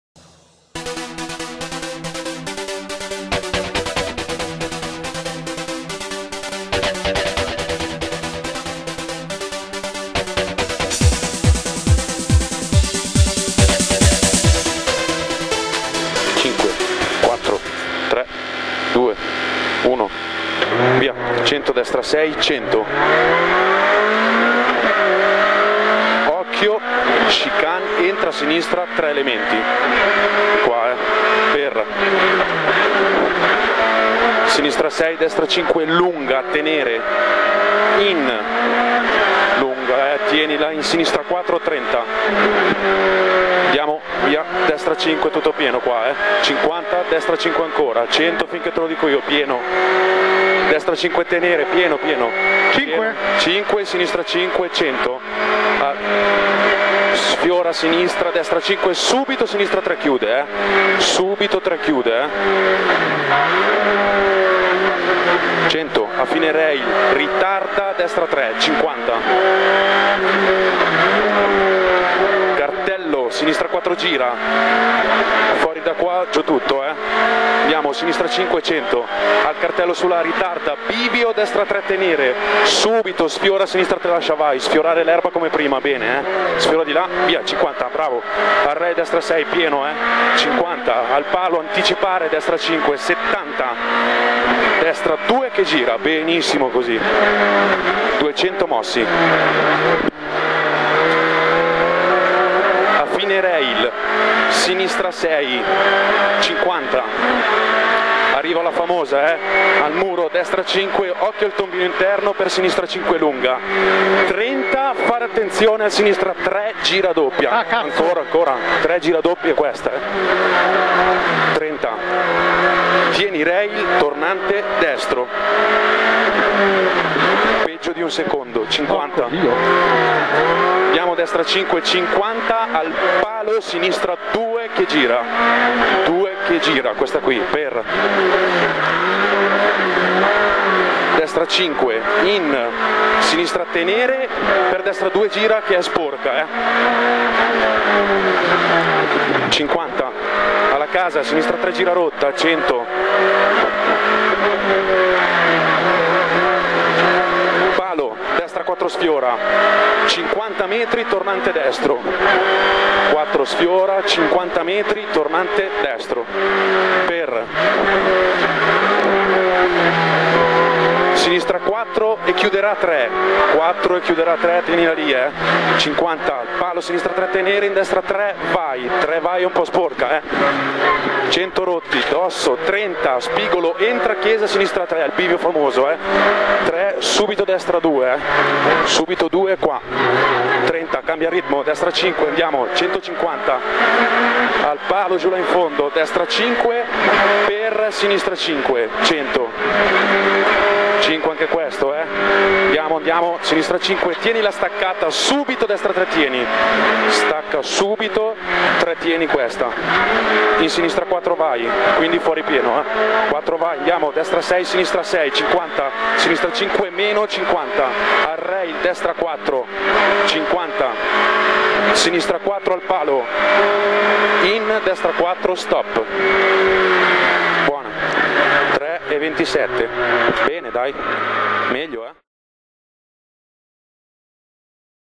9°Rally dell'Ossola 2015 Maci - Rocca by Ferrario Video
Onboard con i Salvatore Maci navigato Daniele Rocca da  su Renault Clio n3 sulla ps 4 Cossogno del 9°Rally dell'Ossola 2015  .
Video realizzato da Ferrario Video